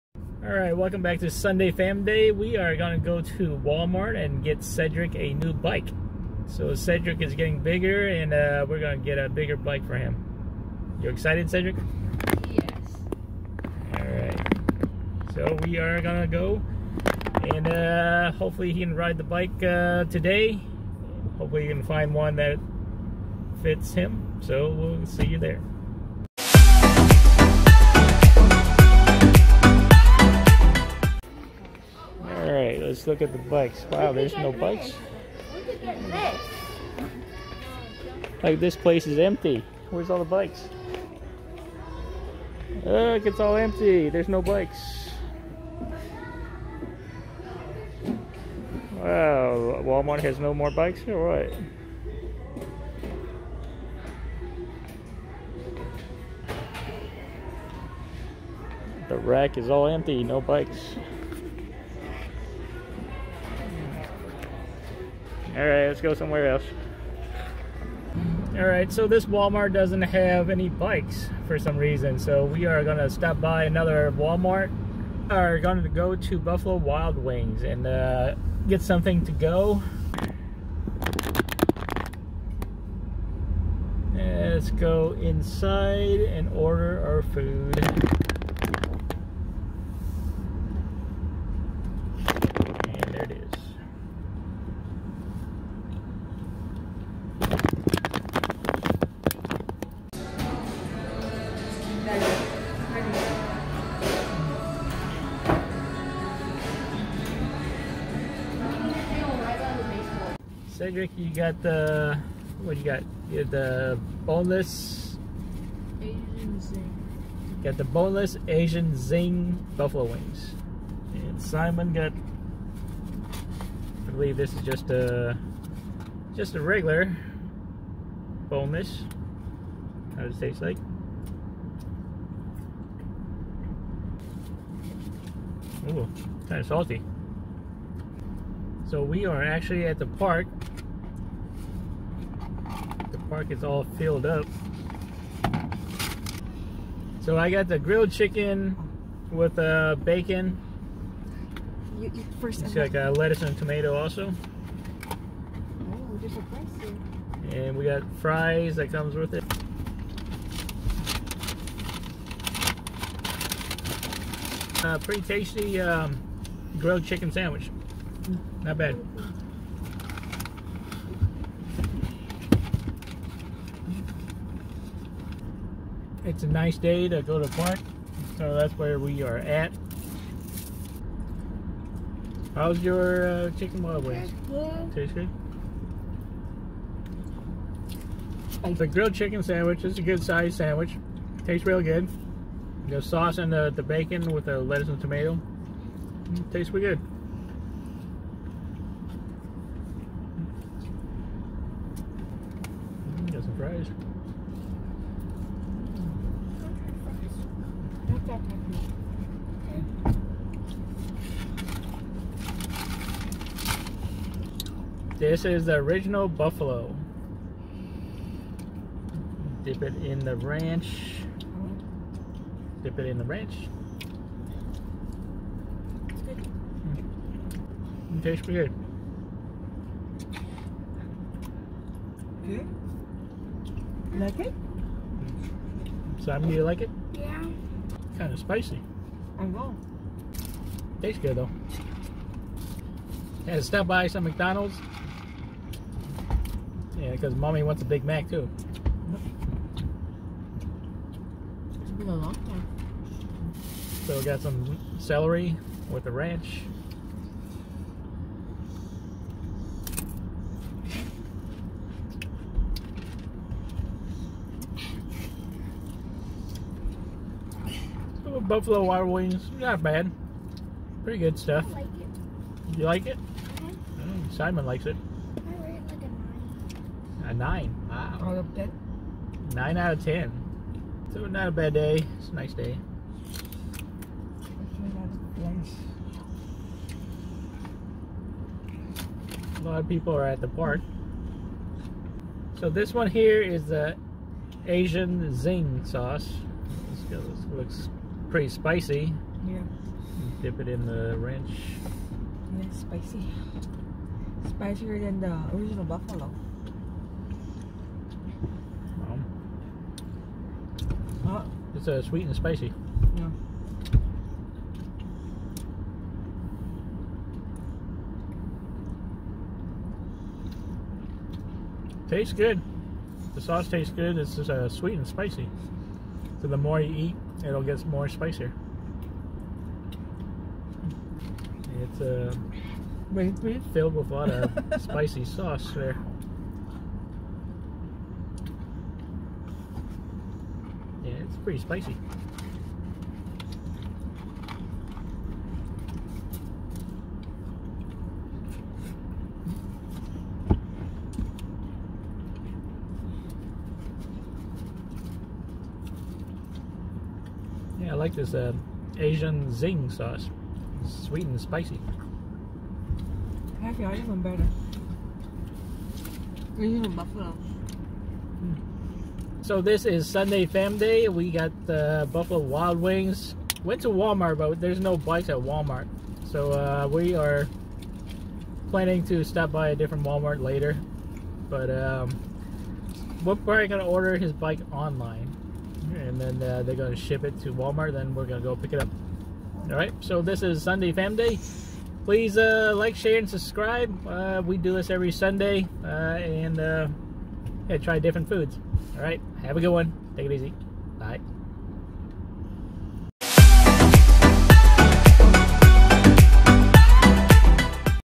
Sunday Famday at Buffalo Wild Wings!
Welcome back to Sunday Famday everyone!
Today we went to Wal-Mart trying to find a new bike, but no bikes at all.. so we stopped by Buffalo Wild Wings to get something to eat! Enjoy!